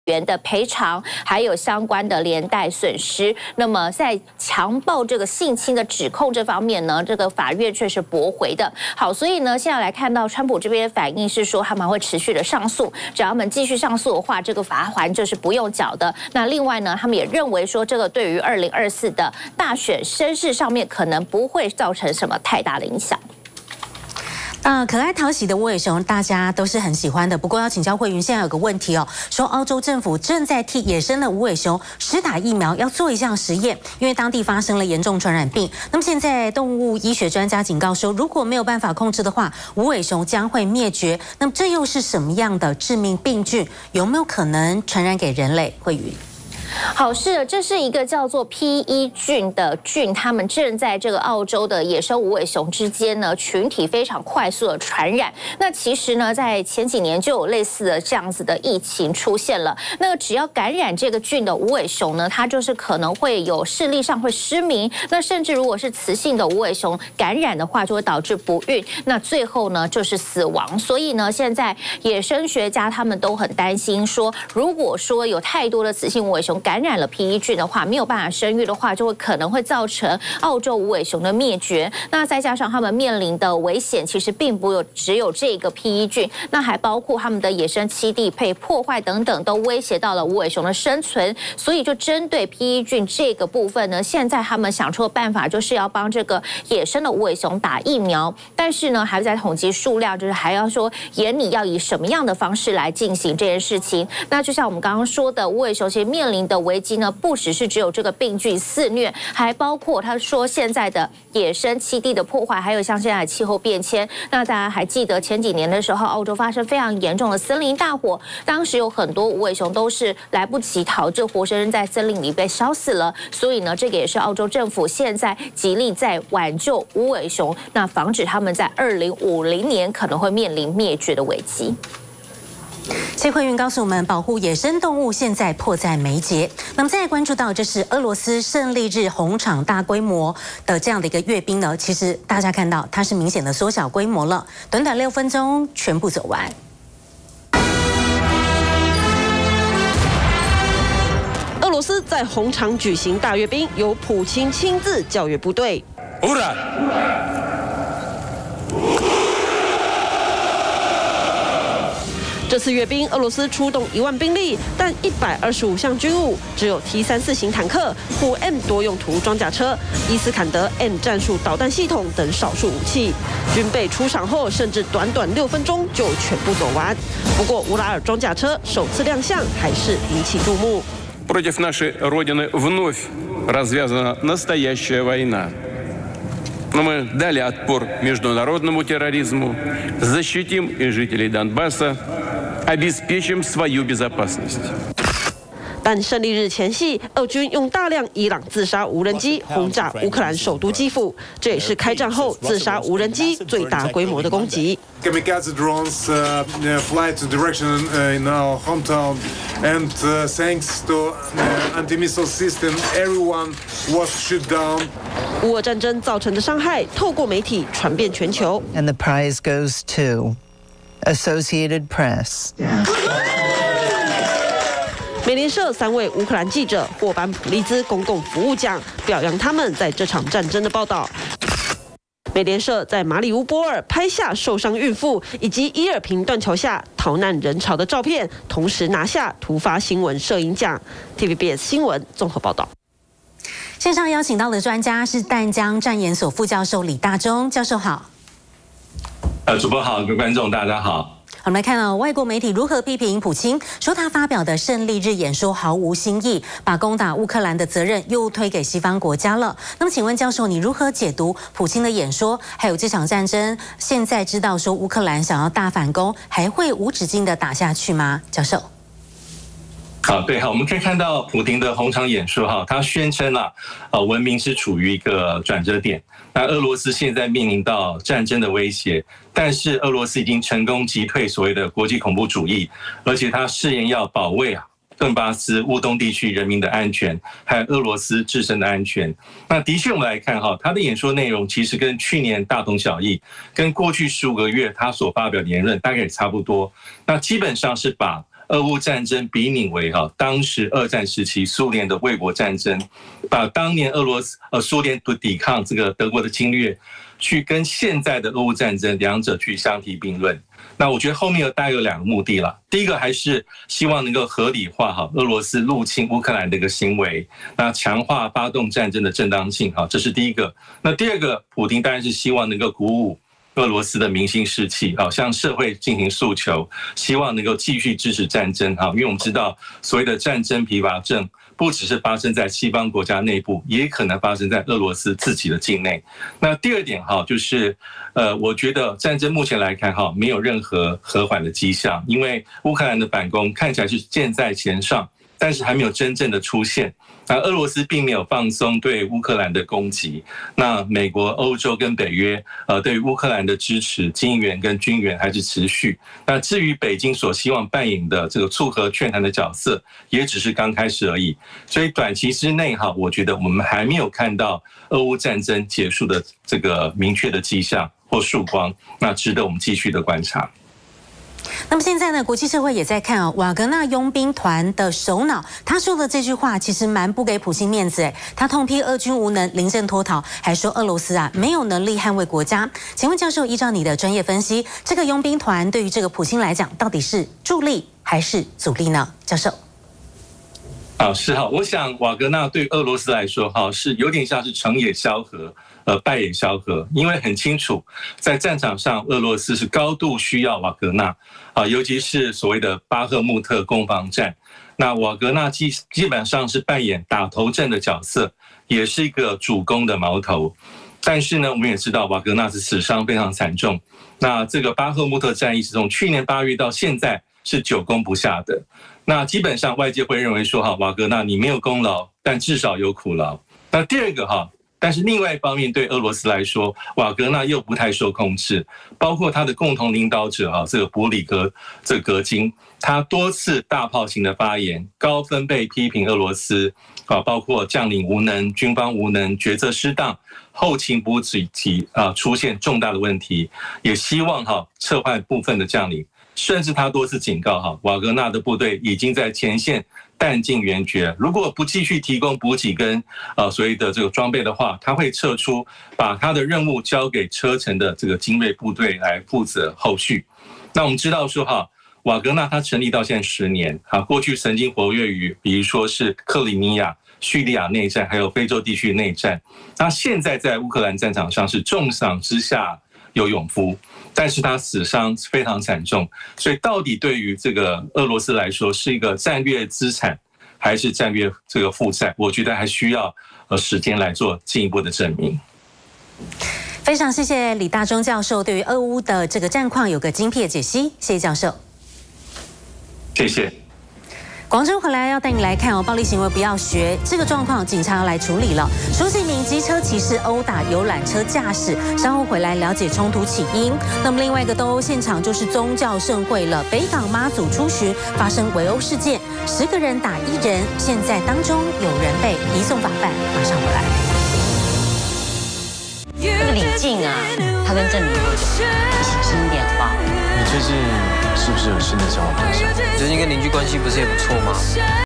陪審團認定川普性侵 判賠500萬美元 俄羅斯「勝利日」外媒：普欽演說無新意LIVE
美國前總統川普，被控告在1990年代性侵專欄作家卡洛爾，陪審團今天判定川普須負法律責任，且須支付卡洛爾500萬美元賠償和懲罰性損害金，這項判決是否會影響川普爭取2024總統大選提名，還有待觀察。另外俄羅斯年度「勝利日」遊行，慶祝納粹德國在第二次世界大戰無條件投降78周年，普欽也公開發表談話，不斷批判西方國家才是烏俄戰爭發動者，外媒指出演說內容毫無新意。